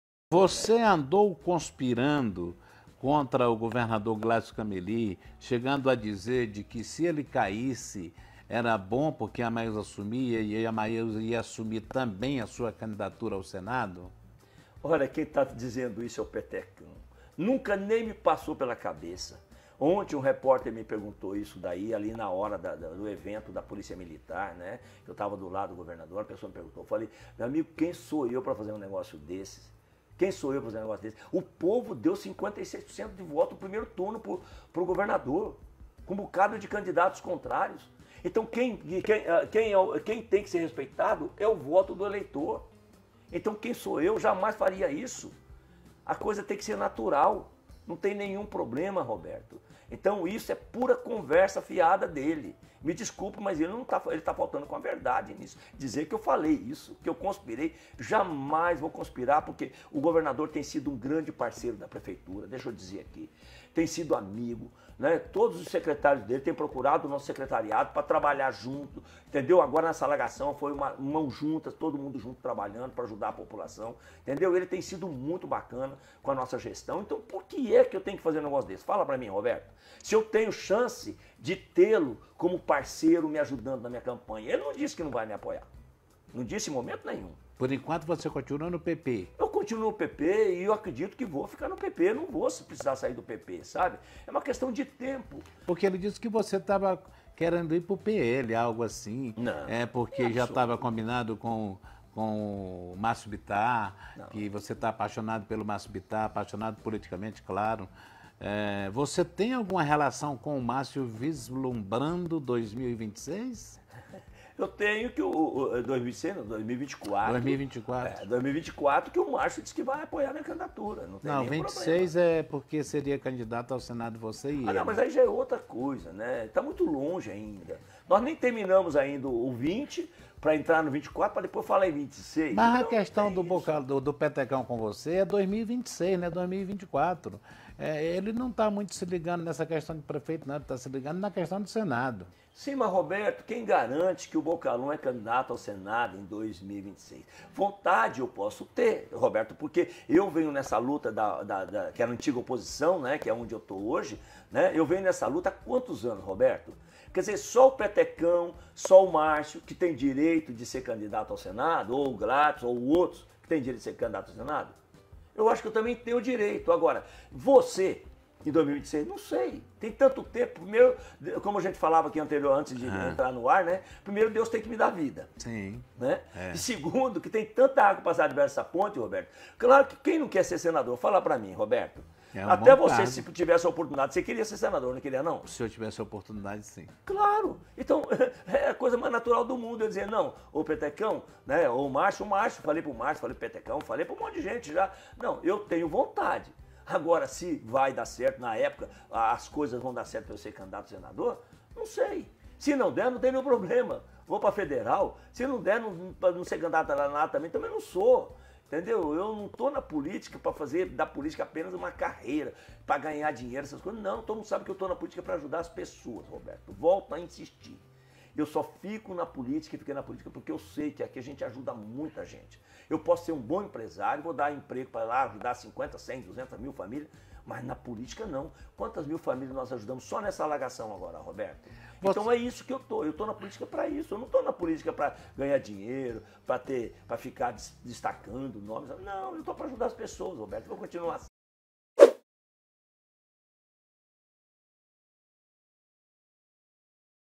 Roberto Vaz pergunta se o Prefeito Tião Bocalom conspirou contra o atual Governador, Gladson Cameli.
Roberto Vaz pergunta se o Prefeito de Rio Branco, Tião Bocalom conspirou contra o atual Governador do Acre, Gladson Cameli.

O Prefeito de Rio Branco, Tião Bocalom fala sobre sobre a conspiração a qual está sendo pautada, contra o atual Governador do Acre, Gladson Cameli. Ressalta ainda sua posição em relação a um apoio ao Senador Márcio Bittar.